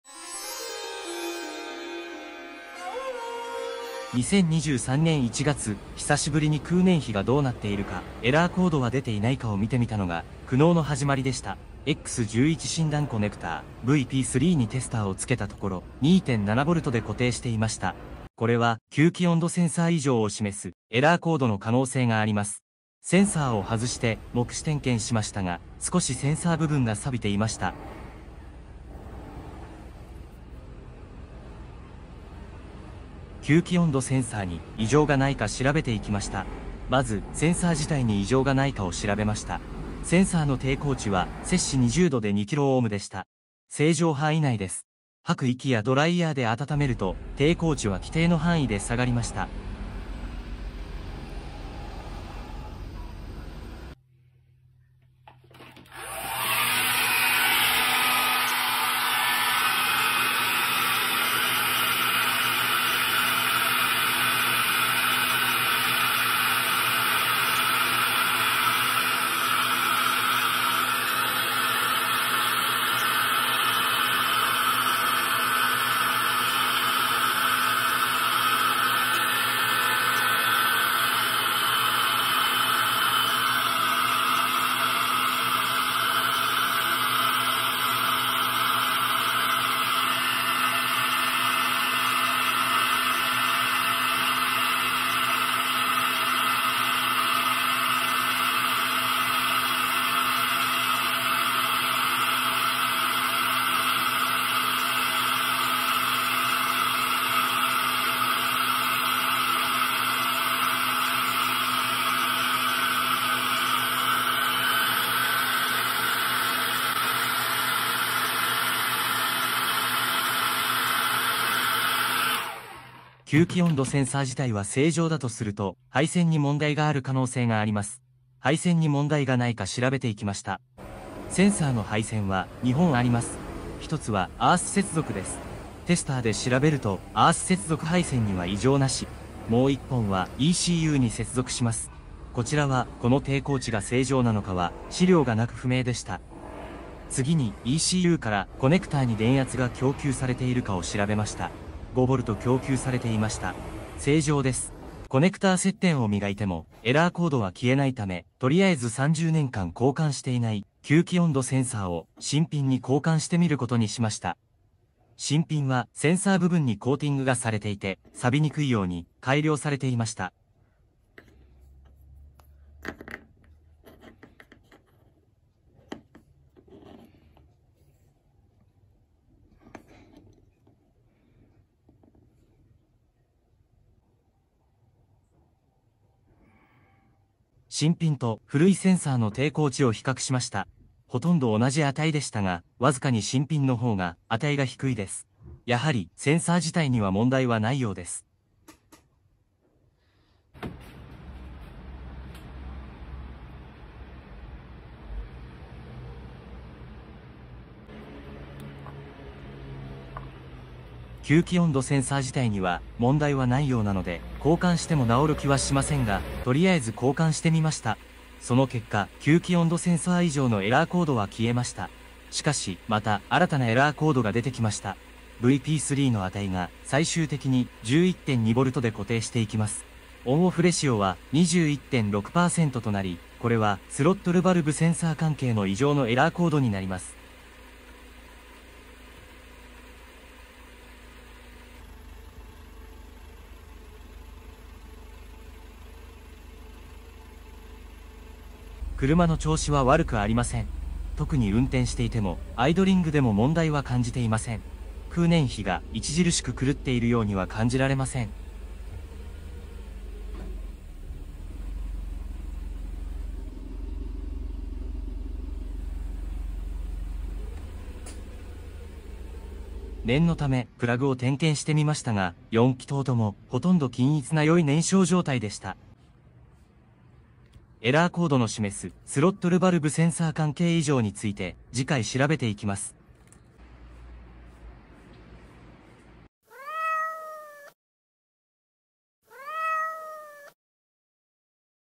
(W201-114)エラーコードとの闘い①吸気温度センサーの点検、交換方法。メルセデスベンツ W201,W124,W126,etc KE model.
KEジェトロシステムのX11診断を見なければ気にならなかったのに、ふと見てしまったために、もしかしたら、とんでもない袋小路に入ってしまったのかもしれません・・・
何かアドバイスがあればよろしくお願いします。